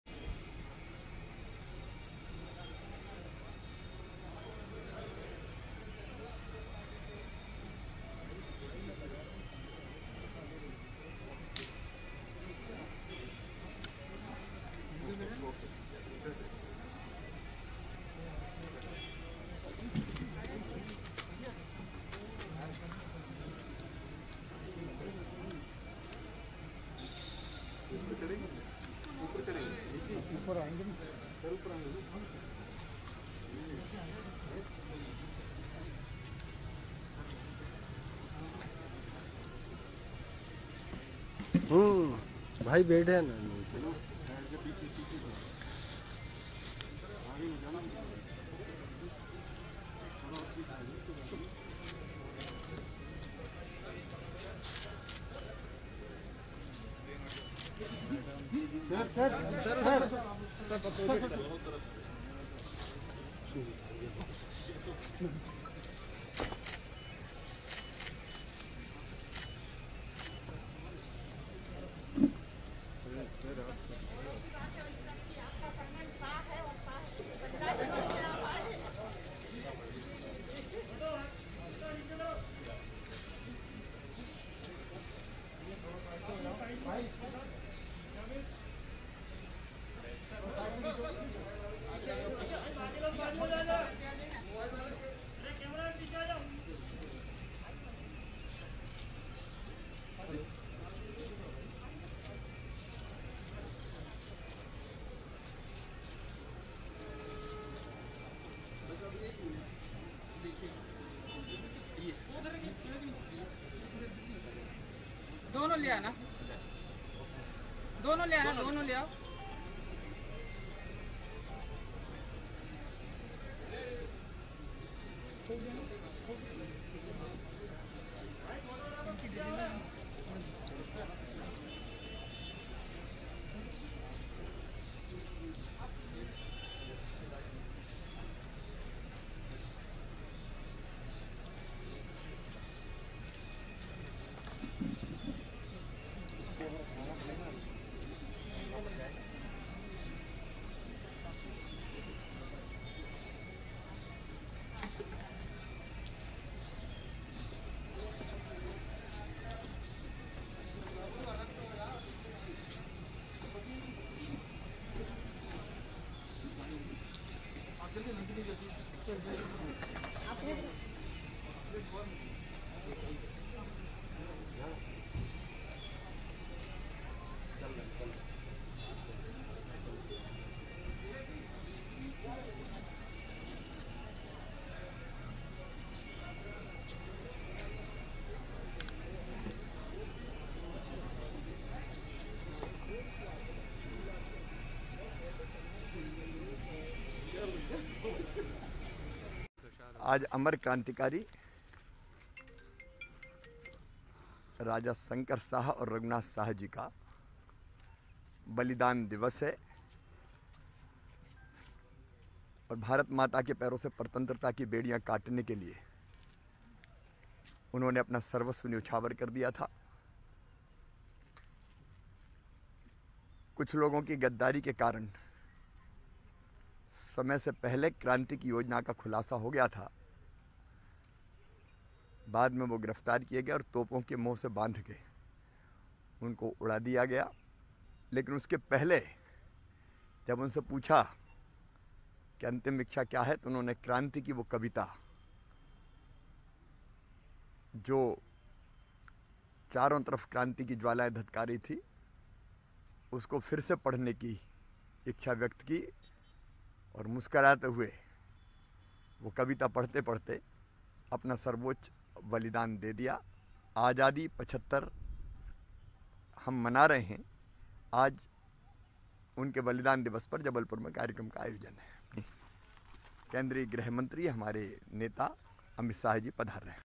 जबलपुर में गृहमंत्री अमित शाह, अमर शहीद राजा शंकर शाह एवं कुंवर रघुनाथ शाह जी के बलिदान दिवस पर LIVE
Jabalpur में अमर शहीद राजा शंकर शाह एवं कुंवर रघुनाथ शाह जी की प्रतिमा पर श्रद्धांजलि कार्यक्रम।

आज अमर क्रांतिकारी राजा शंकर शाह और रघुनाथ शाह जी का बलिदान दिवस है। भारत माता के पैरों से परतंत्रता की बेड़ियाँ काटने के लिए उन्होंने अपना सर्वस्व न्योछावर किया।
AzadiKaAmritMahotsav के अंतर्गत आज उनके बलिदान दिवस पर जबलपुर में कार्यक्रम आयोजित किया गया है।